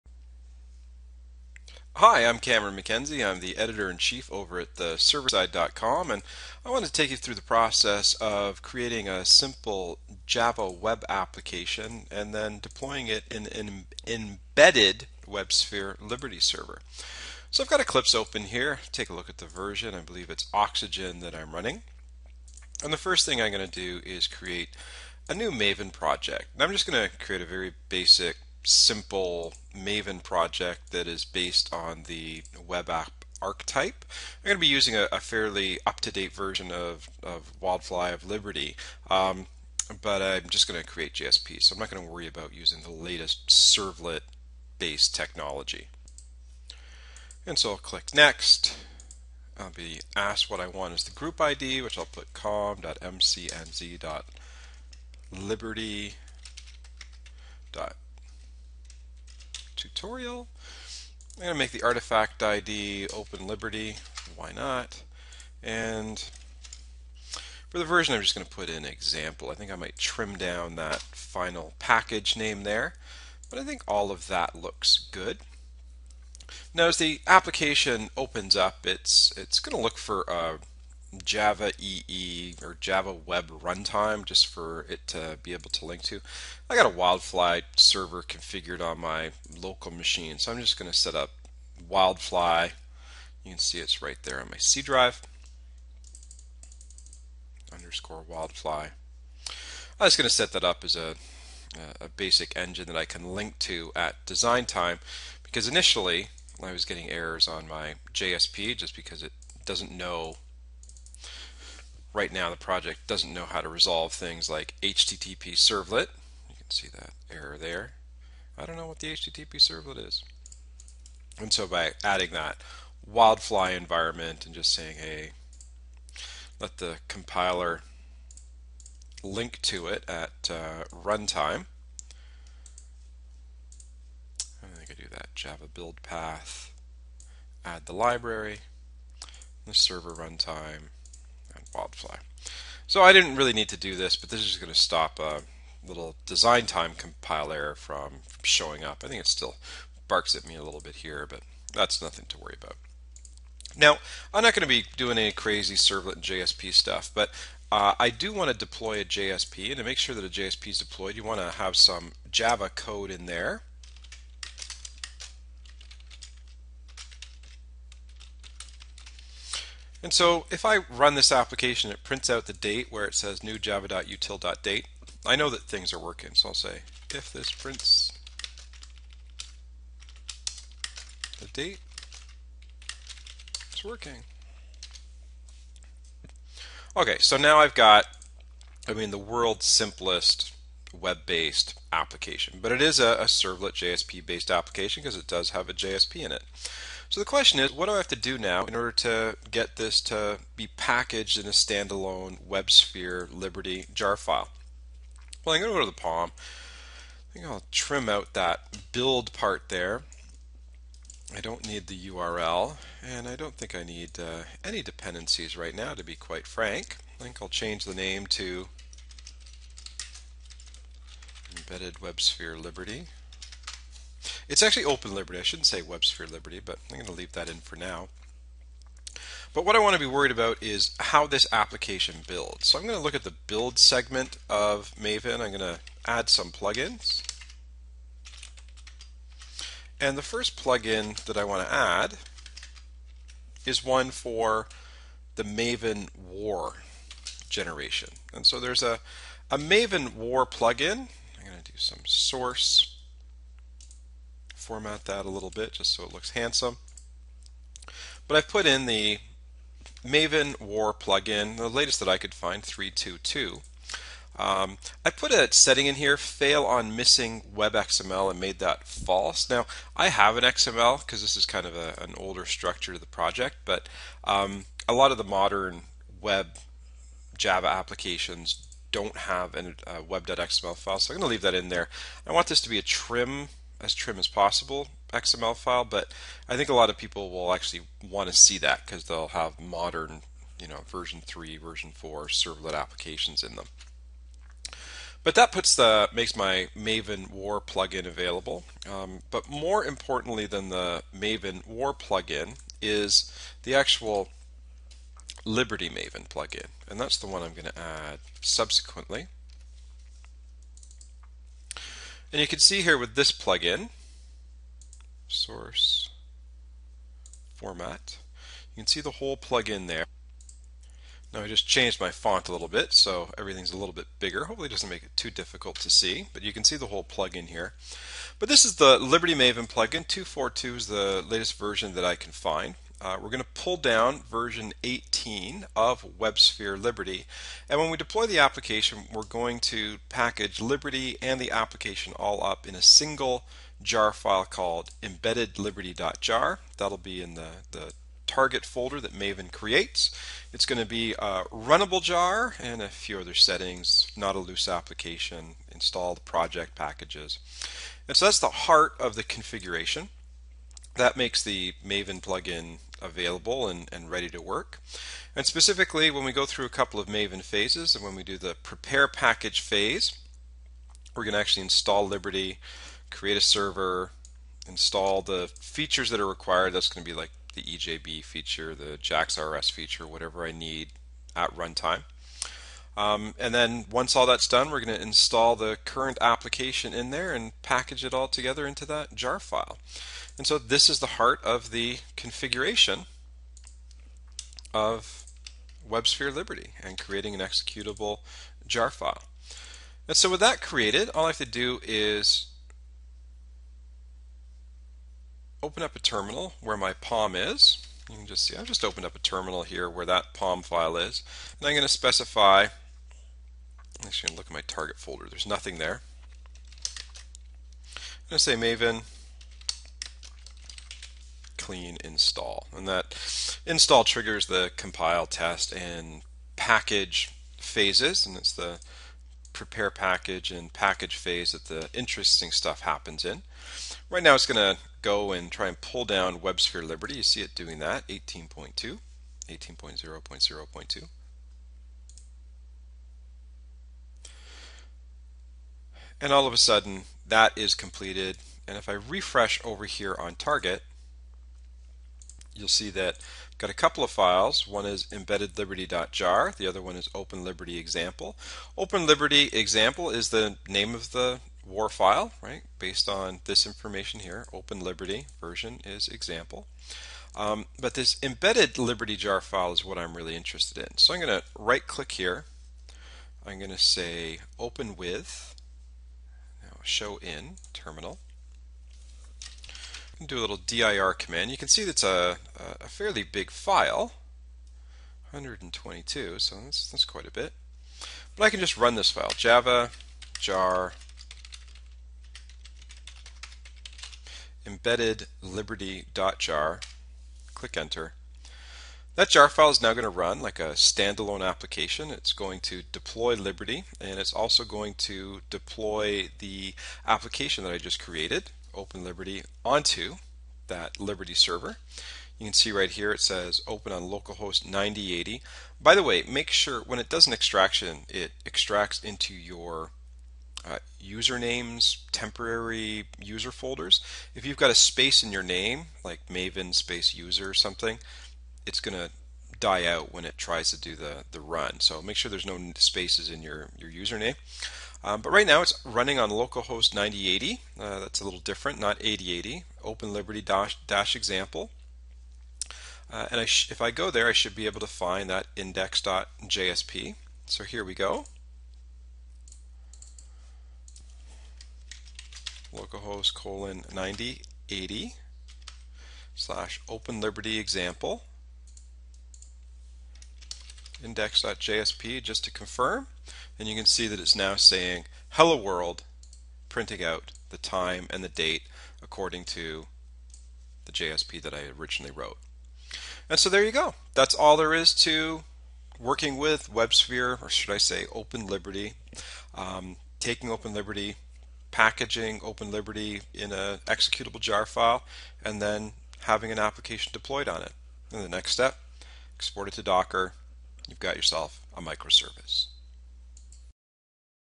Create an embedded Open Liberty executable JAR for WebSphere
This video shows you how to create a runnable JAR file using an embedded WebSphere OpenLiberty applications server. A Java EE web app is deployed to the server,  and the executable JAR is ready for Docker and microservice deployment.